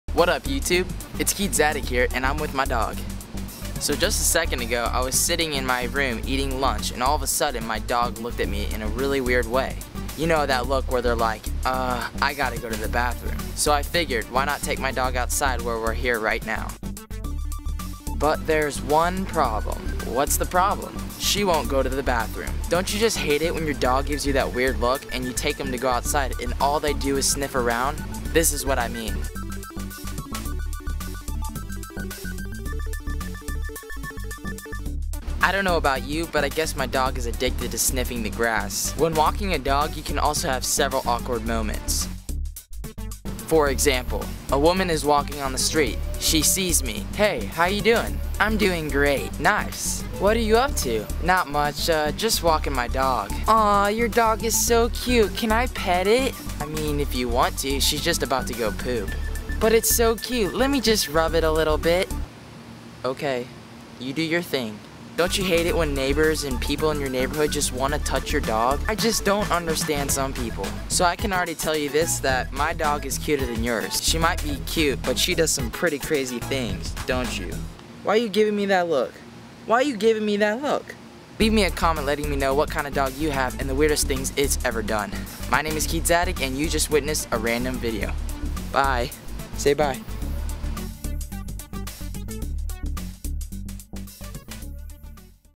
Meet my DOG
Dogs are CUTE, but sometimes they are SO STUPID!
What kind of dog do you have and what is the weirdest thing it's ever done?